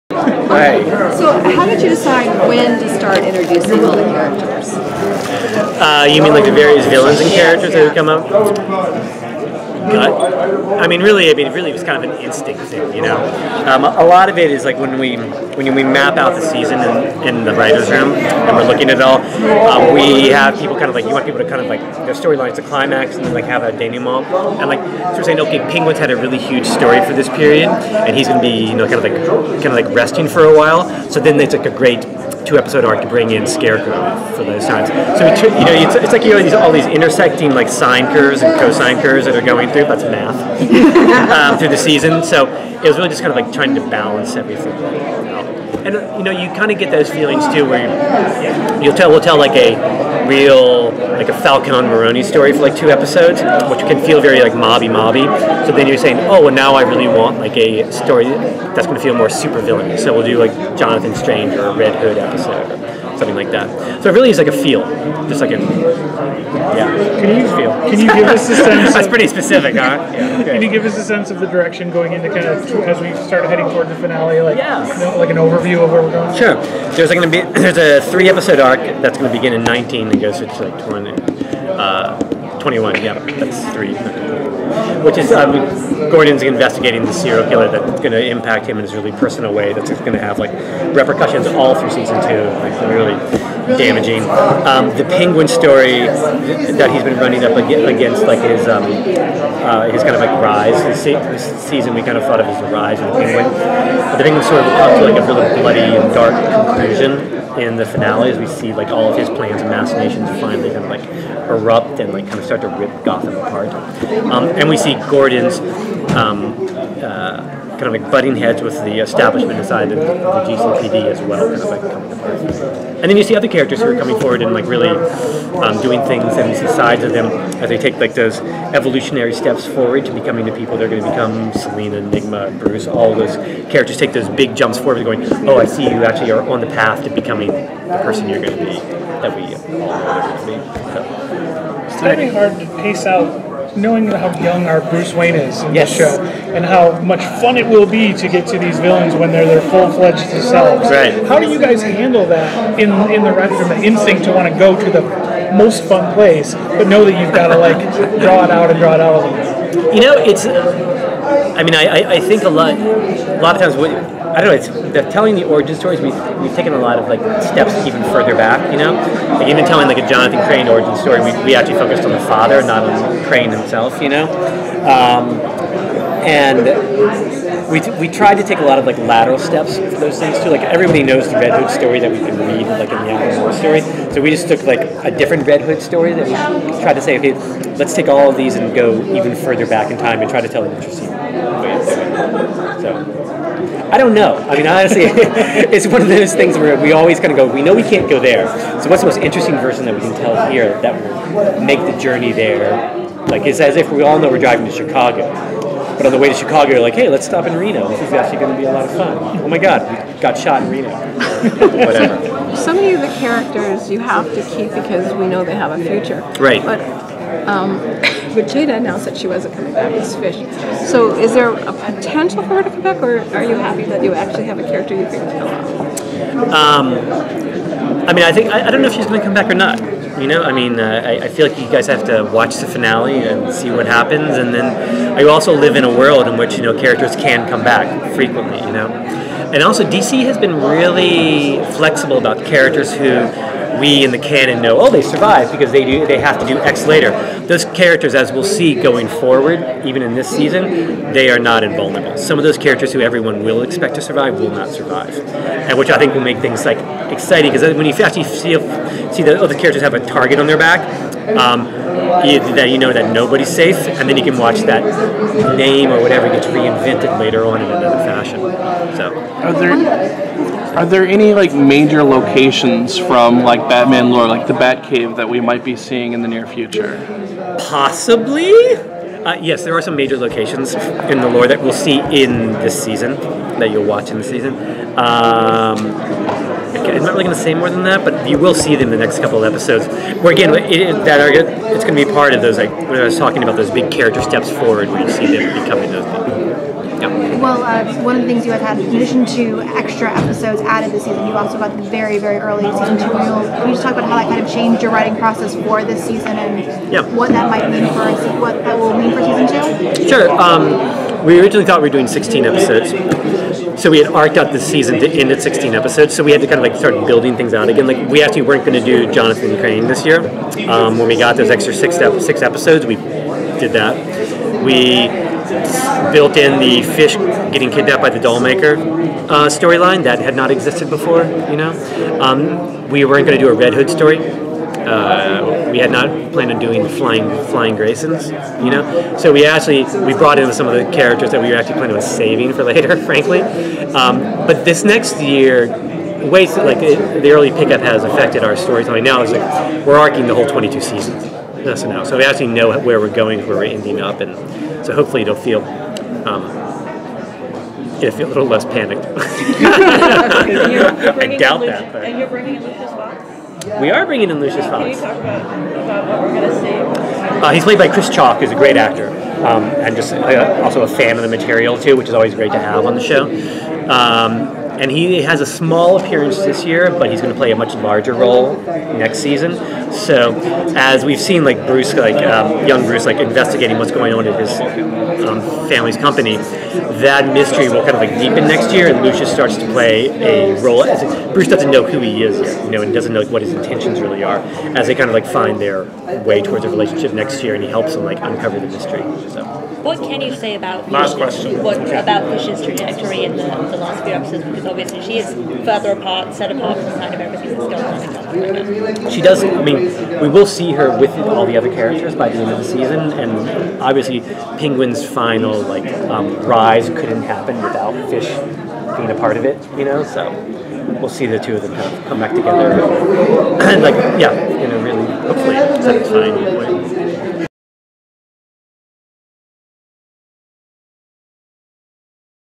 Gotham Executive Producer John Stephens Interview
Interview with John Stephens, Executive Producer of the FOX television series Gotham, done at Wondercon 2015